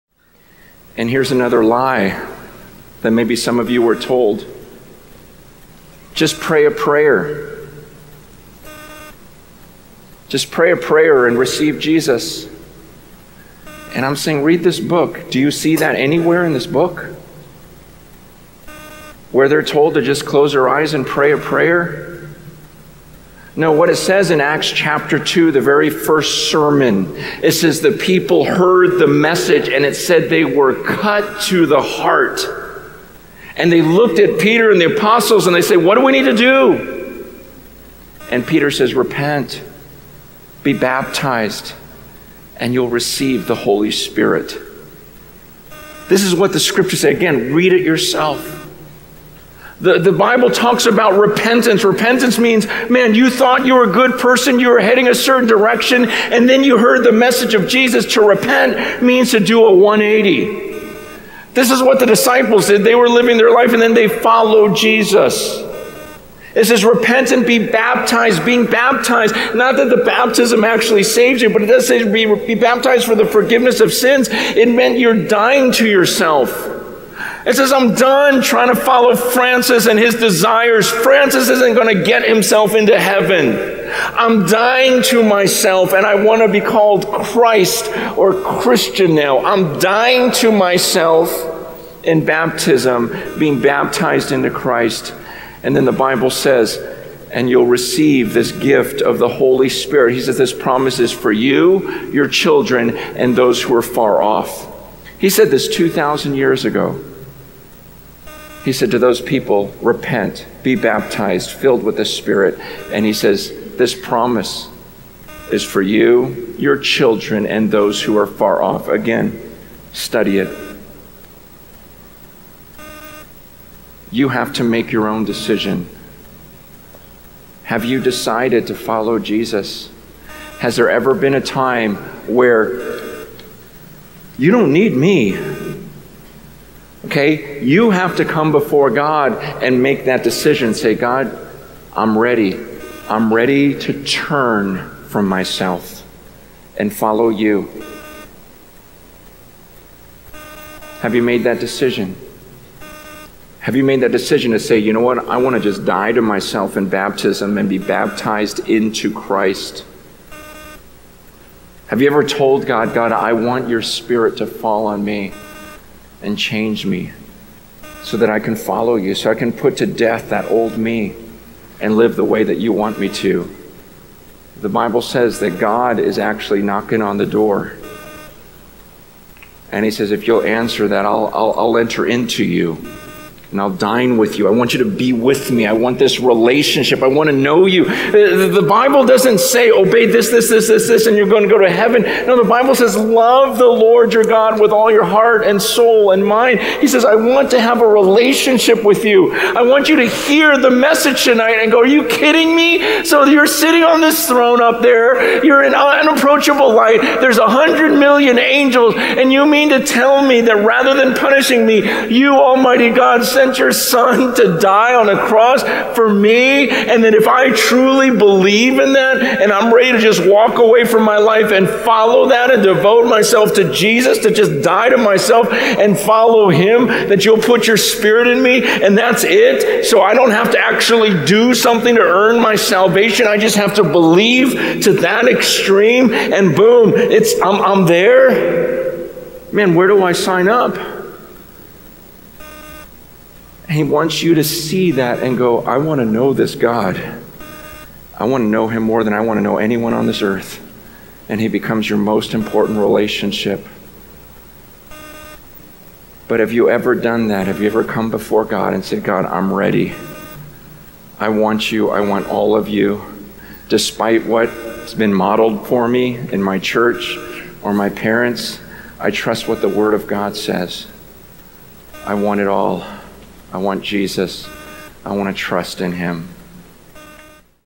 How can I know if my repentance is genuine? What is a true repentance? - Francis Chan
Francis Chan - How can I know if my repentance is genuine? What is a true repentance?
What true repentance means?
What is repentance according to the Bible?
What is repentance in Christianity?
Francis Chan explains the true meaning of repentance and what does it means to repent, and how to check if you have repented.